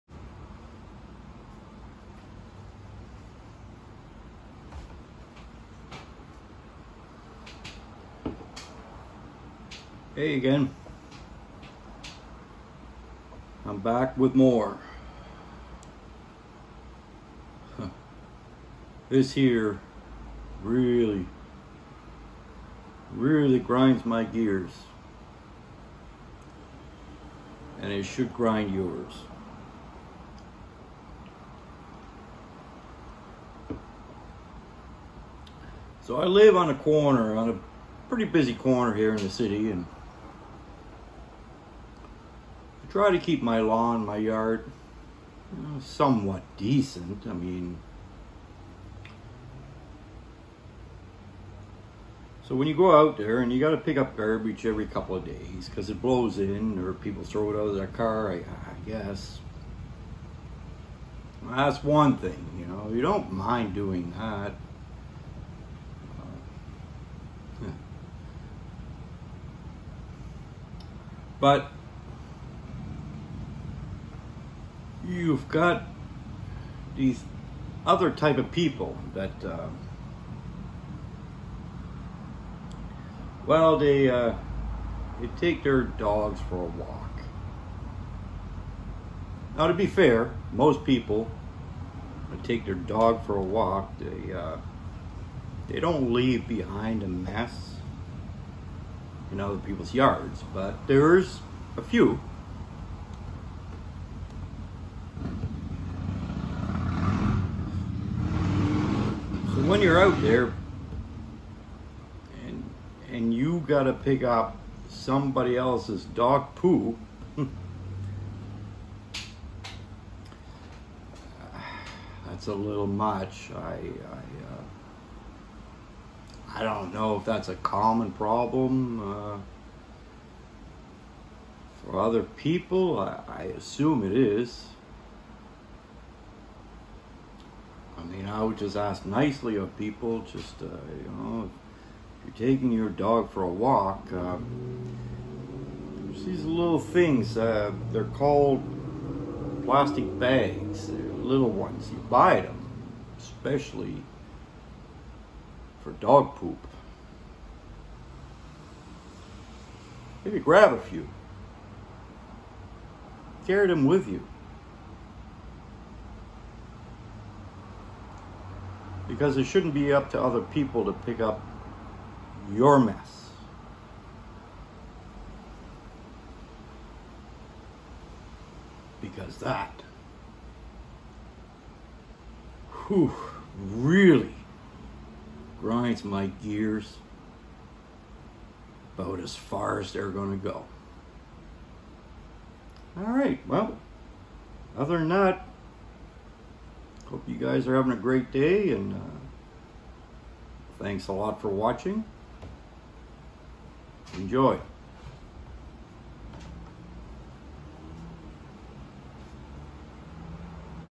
This Really Grinds My Gears Episode 4 "The Dog Pooh" Just Another Rant About Stupid Things
This Really Grinds My Gears Episode 4 "The Dog Pooh"
Just Another Rant About Stupid Things

People, you can buy these things called doggy pooh bags, I suggest you invest in these as they are not expensive and readily available, Thanks